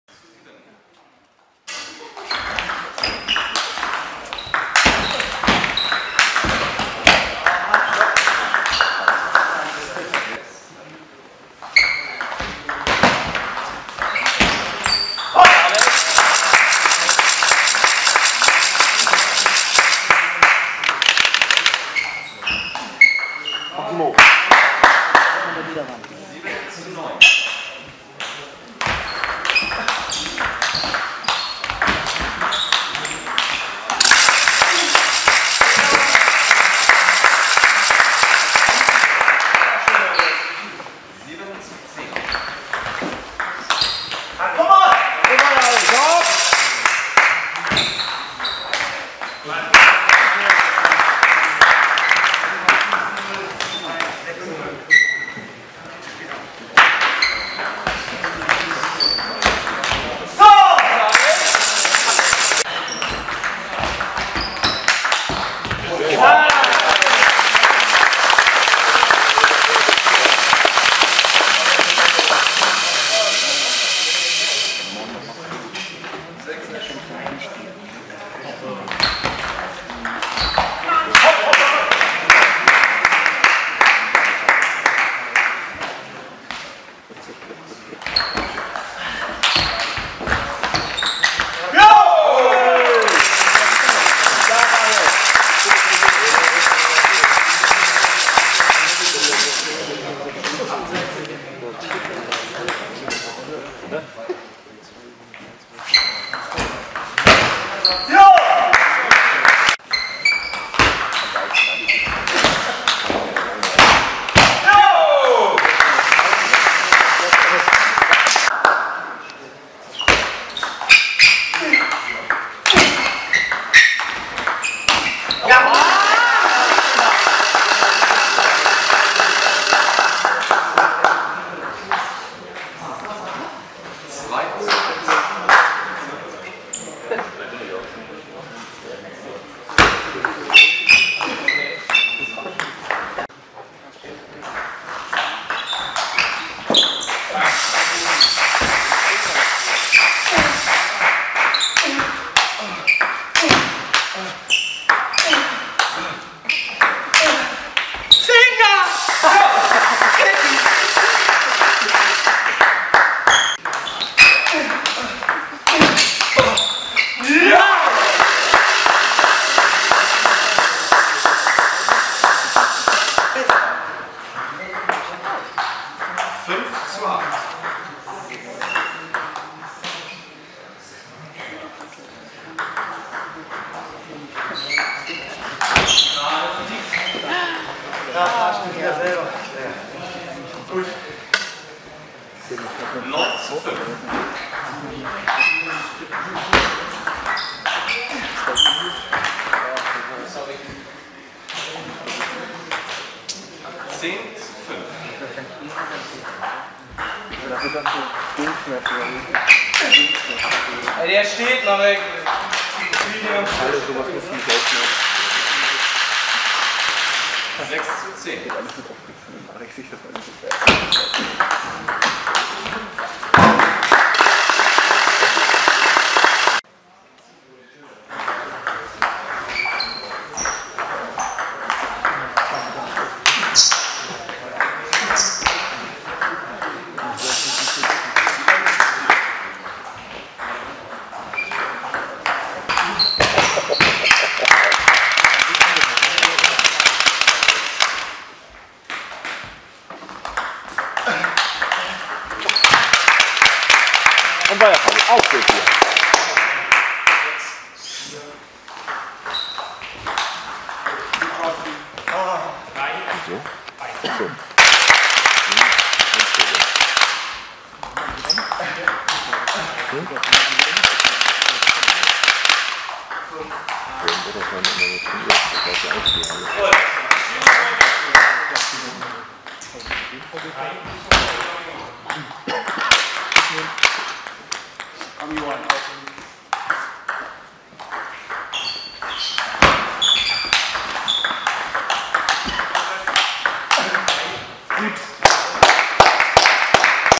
2_Herren_MTV_Jever
Einige Ballwechsel der 2. Herrenmannschaft des MTV Jever von den Relgationsspielen zur Verbandsliga gegen den TSV Lunestedt (9:3) und den ESV Lüneburg (9:4)
Damit steigen die 2. Herren des MTV im Tischtennis-Verband Niedersachsen in der Saison 2012/13 in die Verbandsliga Herren Nord auf !